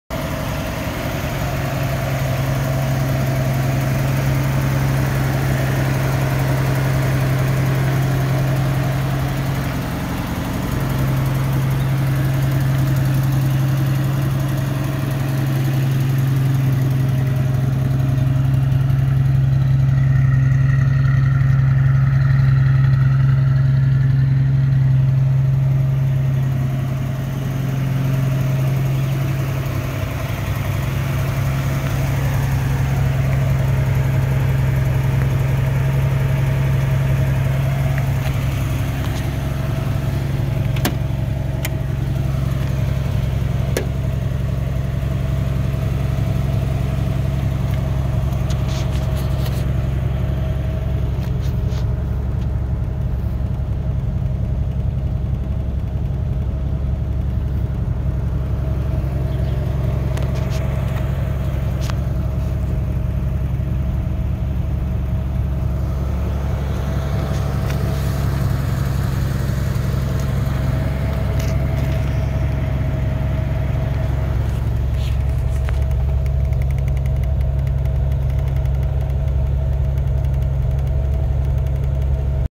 Hudson Hornet Taxi Walk Around
Walk around video of a hudson hornet taxi cab that was recently restored.